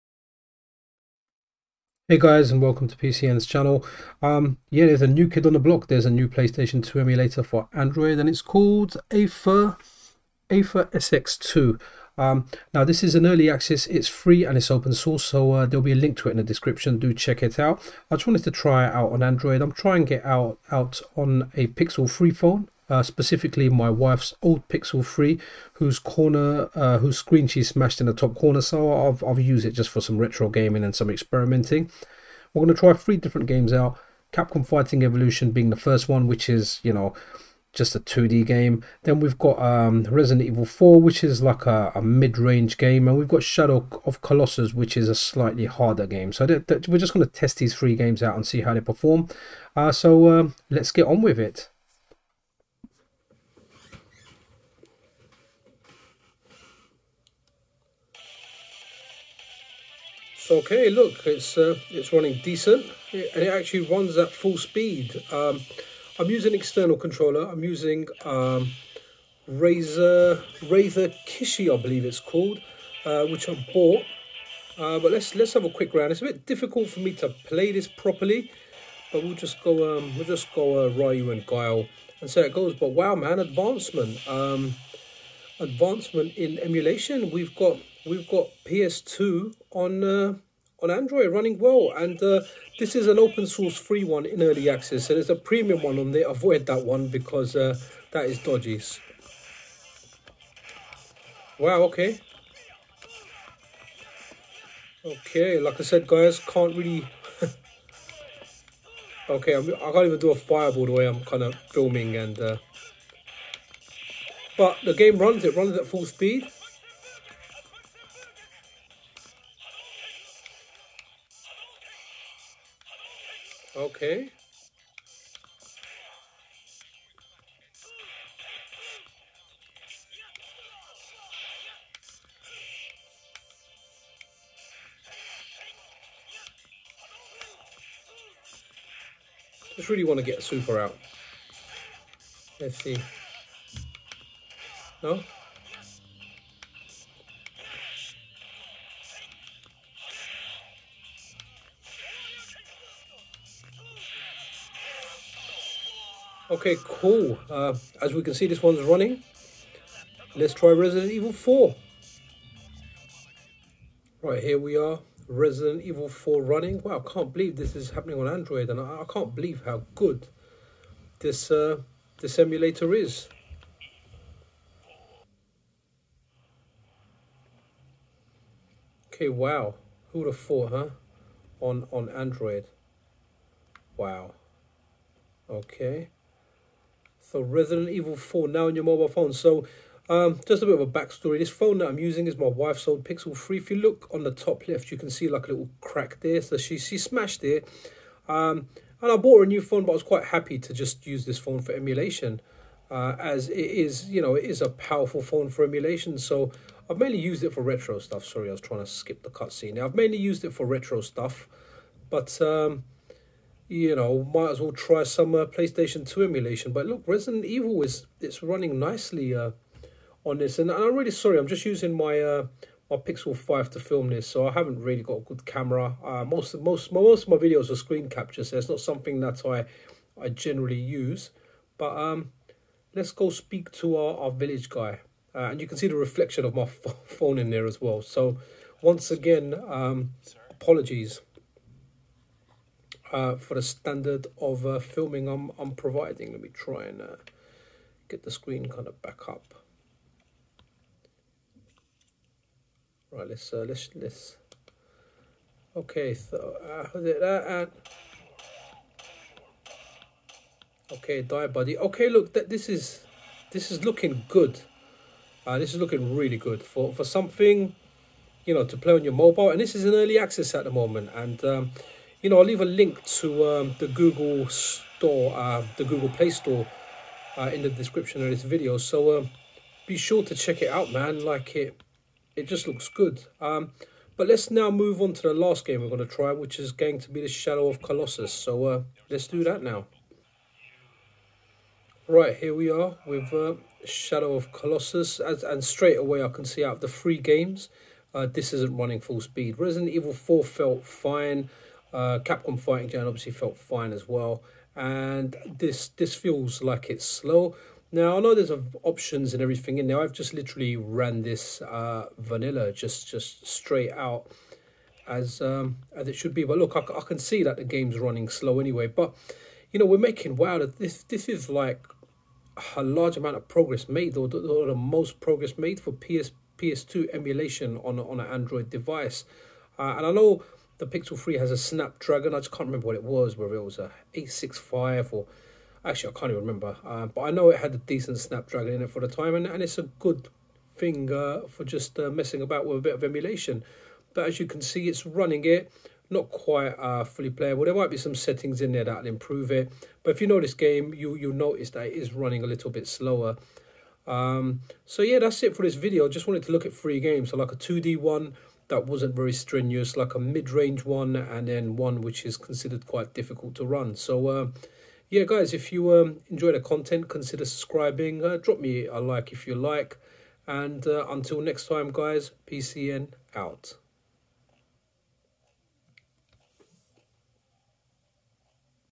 Play PlayStation 2 Games on your Android device AetherSX2 Early Access Emulator - Tested on Pixel 3
Please note - I stated in the video that the emulator is open source. This is incorrect. Apologies for that.

So guys, we finally have a free emulator for the Sony PlayStation 2. There are some dodgy premium ones on the Play Store, please avoid those and only stick with AetherSX2.

Tested using a Google Pixel 3 Android phone and a Razer Kishi controller.